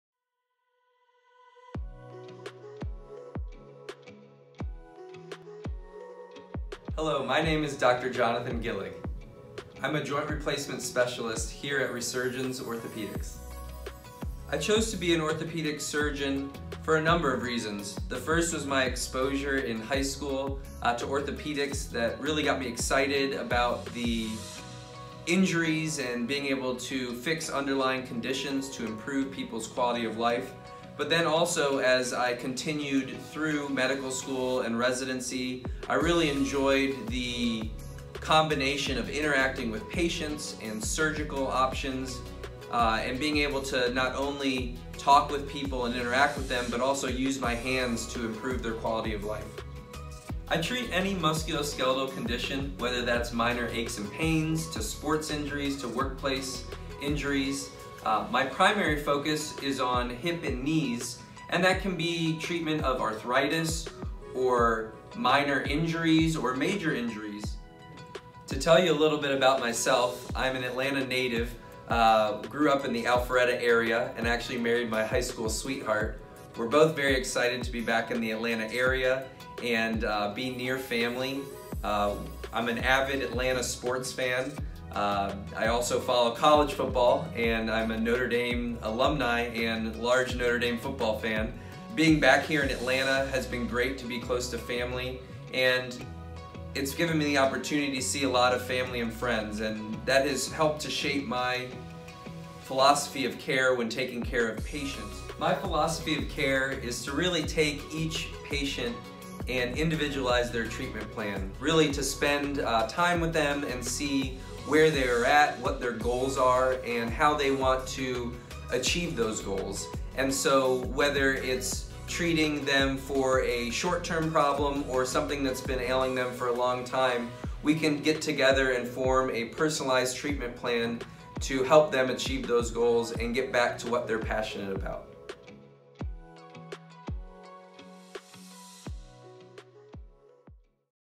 Dr. Jonathan Gillig – Joint Replacement and Knee & Hip Arthritis
Dr. Jonathan Gillig is a Fellowship-Trained Joint Replacement expert specializing in knee and hip arthritis, knee and hip injuries and ailments.